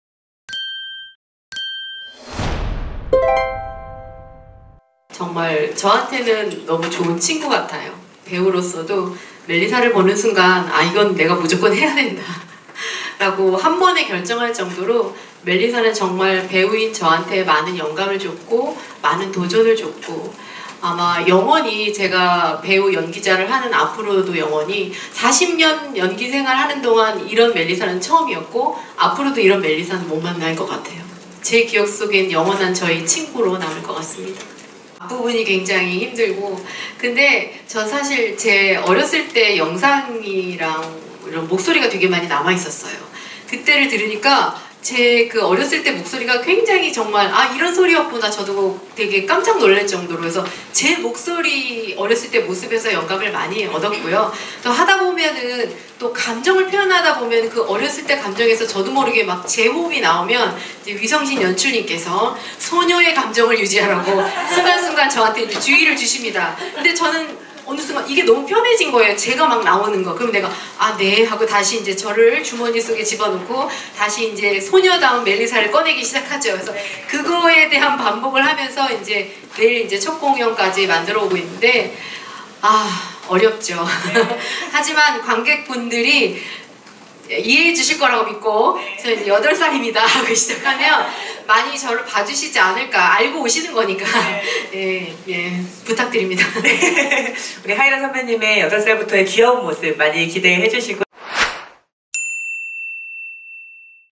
배우 하희라, 기억 속에 영원한 친구로 남을 것 연극 '러브레터'
2022년 9월 22일 프레스콜 현장